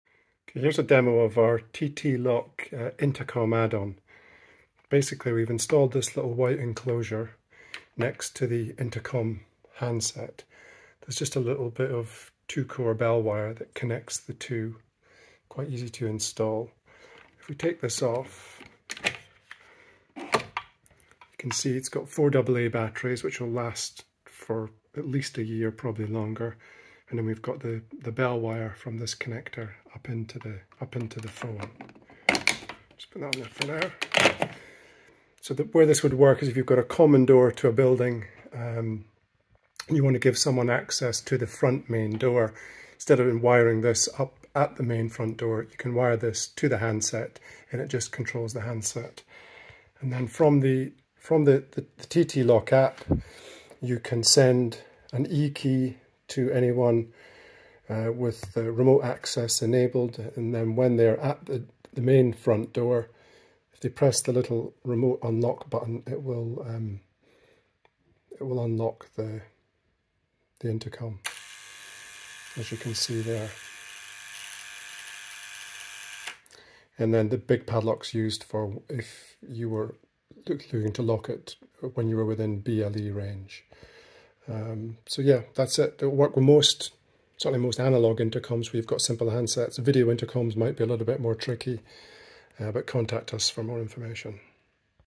TTlock Intercom controller
This short video explains how the TTLock intercom controller connects to the intercom handset and enables remote access to the main common door. Requires the TTLock Wi-Fi gateway/bridge.